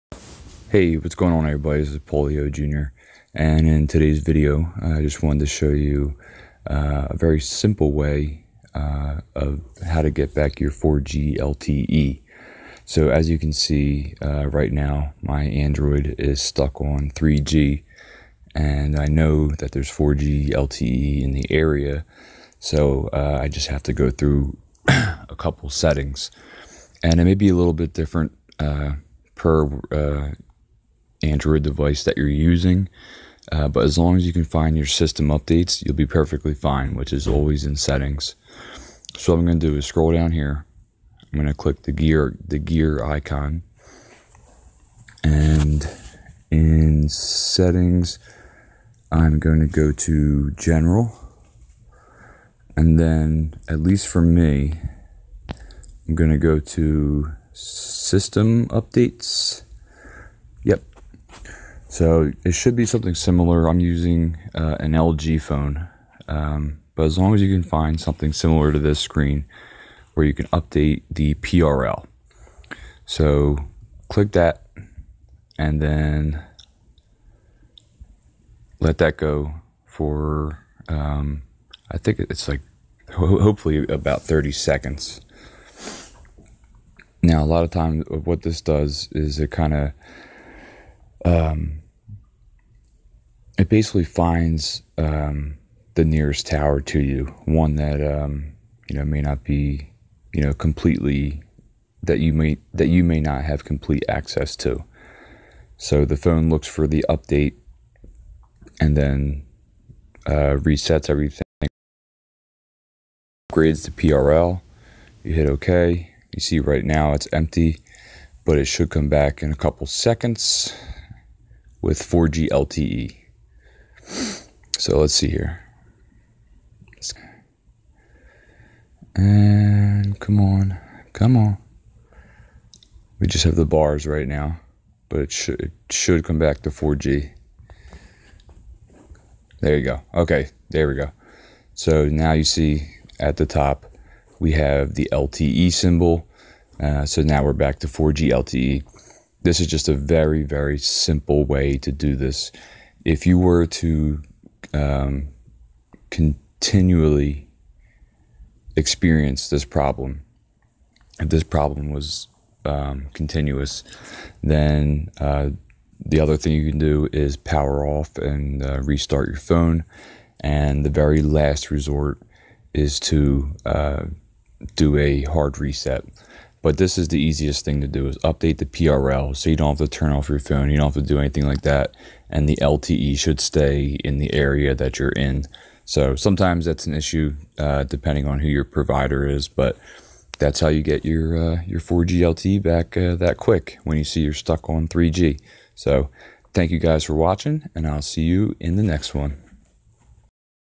Phone Won't Connect to 4G LTE Network - ANY Android & Samsung - SOLVED 2019
This is a short step-by-step tutorial when you've noticed you are only getting 3g and not 4G LTE.  This process will also work if your mobile data isn't working at all. I hope this helps!

If I have helped you in any way please make a small donation. ANY amount IS MUCH APPRECIATED!